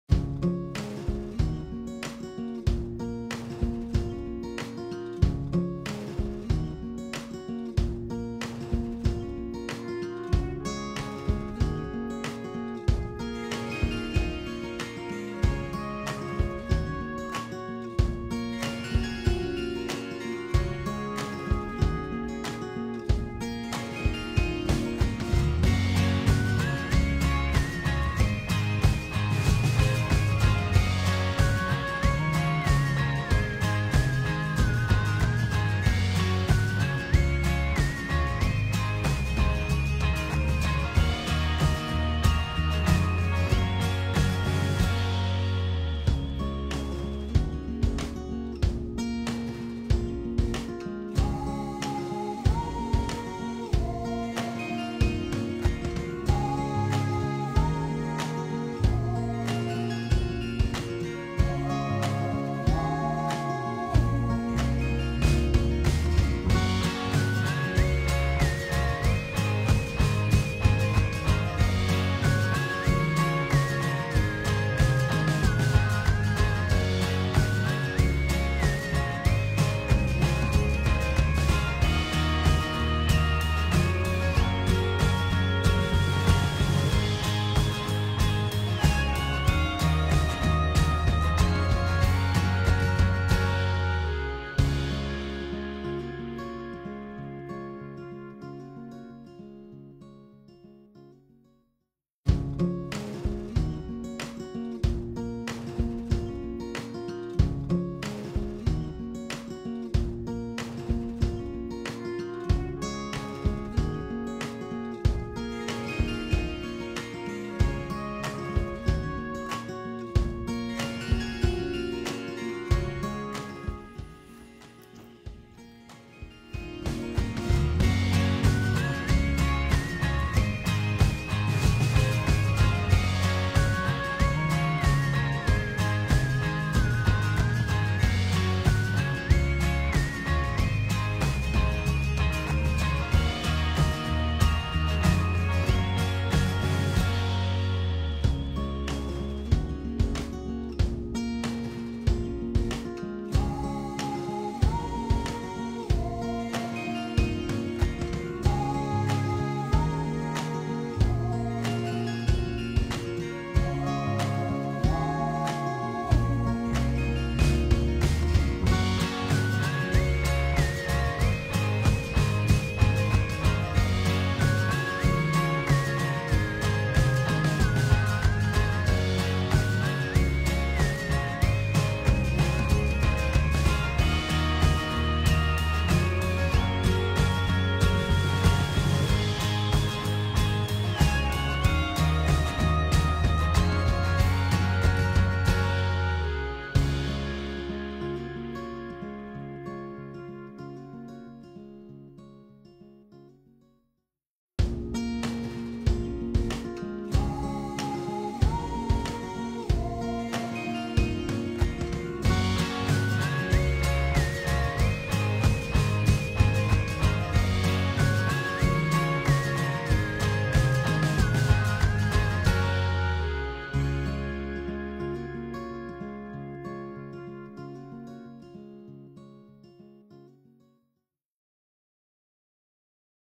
How to clean a rabbit enclosure 🐰: tutorial
In this video we will show you how to clean up a rabbit enclosure